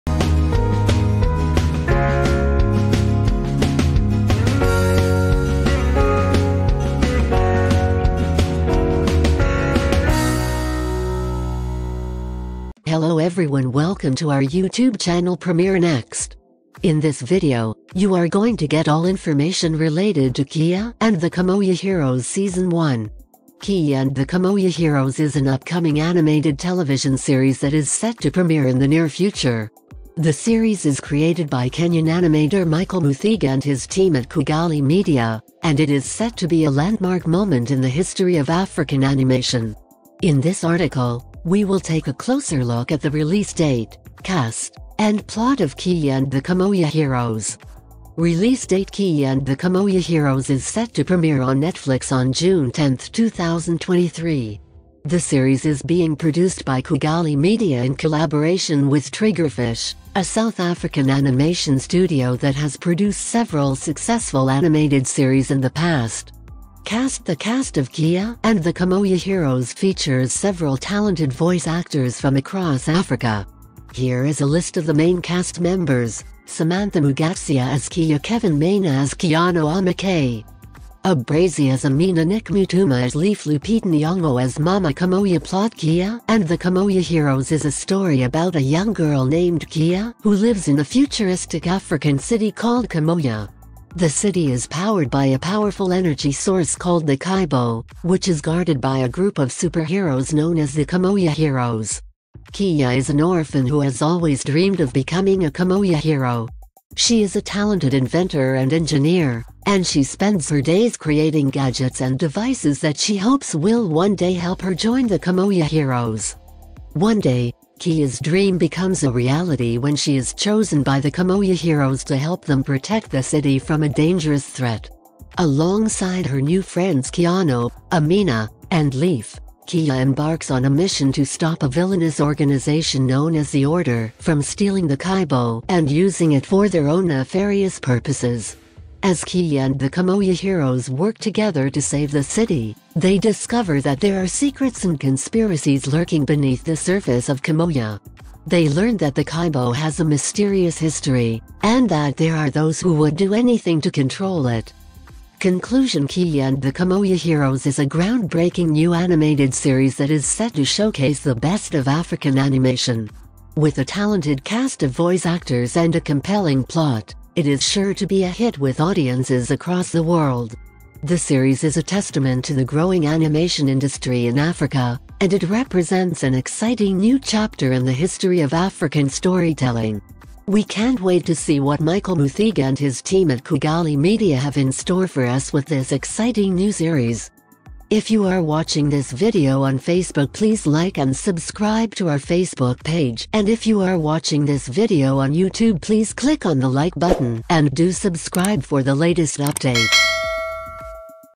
Kiya and Kimoja Heroes Season 1: Coming To Soon On Disney+ - Premiere Next
Kiya and Kimoja Heroes Season 1: Coming To Soon On Disney+
in this video, you will get the info related to Kiya and Kimoja Heroes Season 1 release date, cast, and key details.

Technology updates
Netflix Series
Netflix Movies
Premiere Next